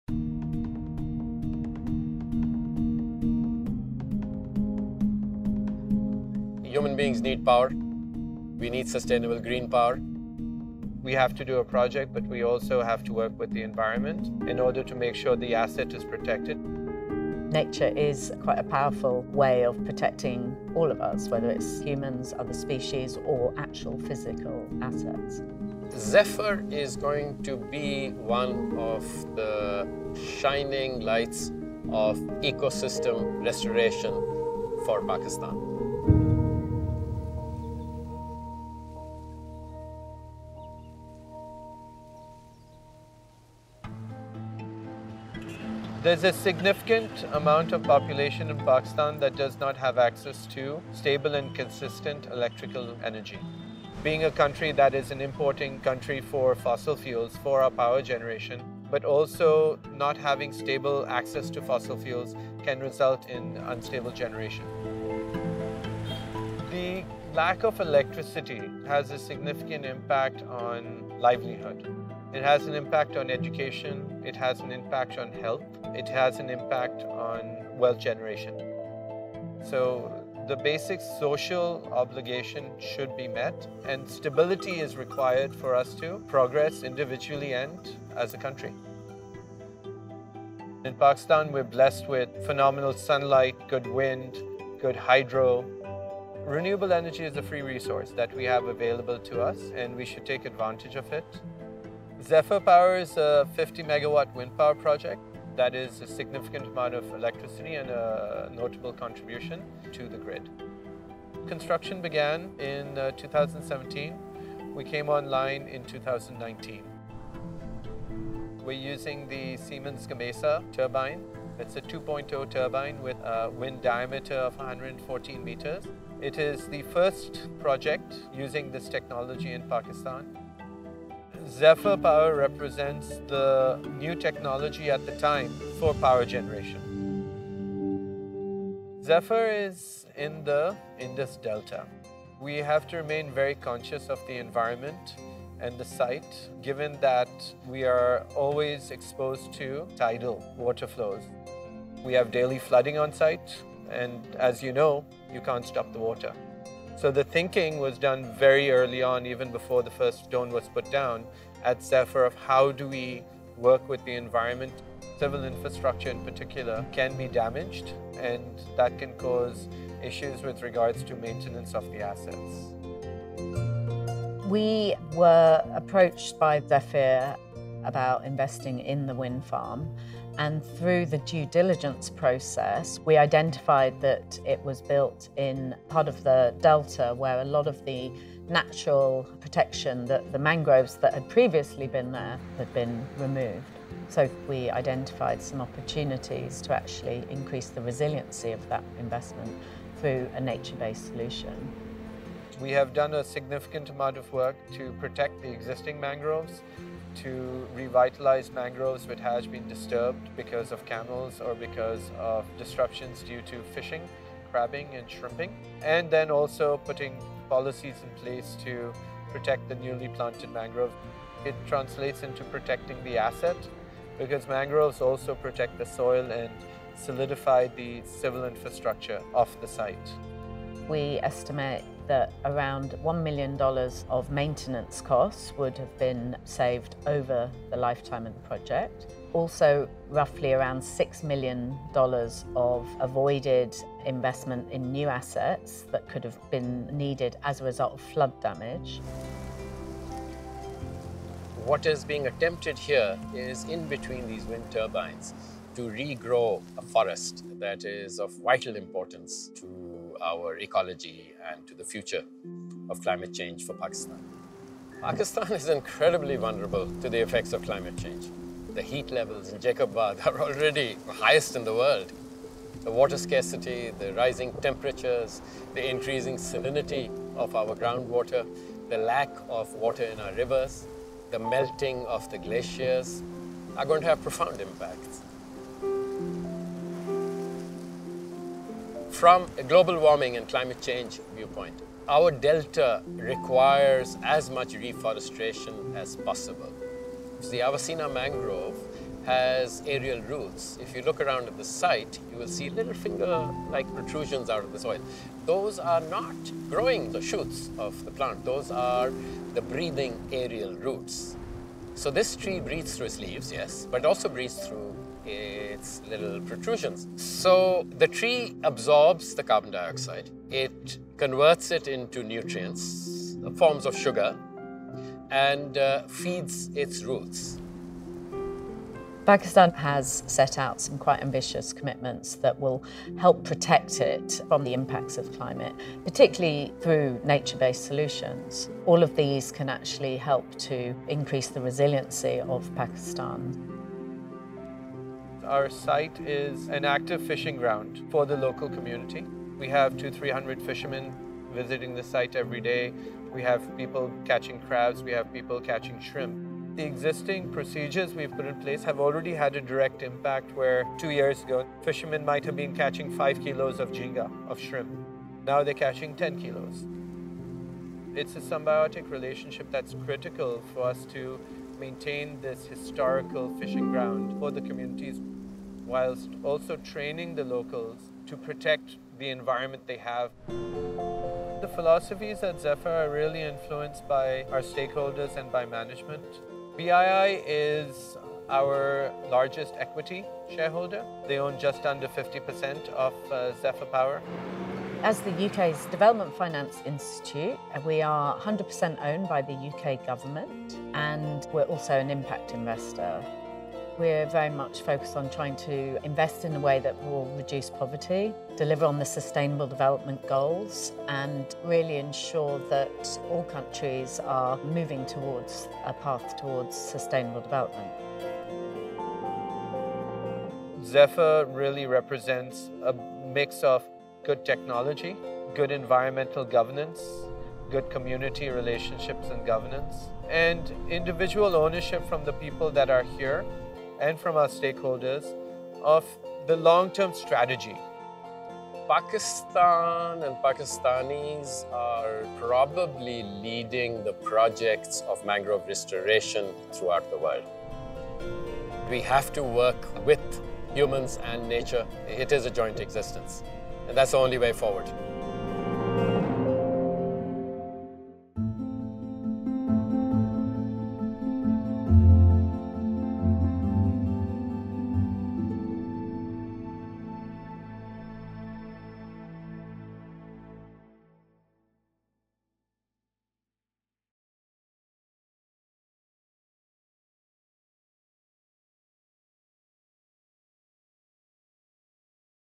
Financing Nature Restoration With Clean Energy
Zephyr Power's wind turbines near Karachi provide much-needed renewable energy in Pakistan at the same time as restoring local mangroves; a Nature Based Solution that helps reduce the devastating impact of floods in the Indus delta and draw down CO2 from the atmosphere.

Kumayl Khaleeli, CEO of Zephyr Power, explains how this 50KW project harnesses Pakistan's abundant natural resources to point the way towards clean energy security for communities across the country, improving livelihoods and reducing dependency on imported fossil fuels.

Amal-Lee Amin, Managing Director Climate, Gender & Diversity and Advisory at British International Investment outlines how this project fits into the broader context of aligning Nature Based Solutions and renewable energy generation.

Architect and Environmentalist Tariq Alexander Qaiser reveals that the restoration of the mangrove ecosystem in between the turbines not only helps with natural flood defences and encourages biodiversity, but also absorbs carbon dioxide from the atmosphere.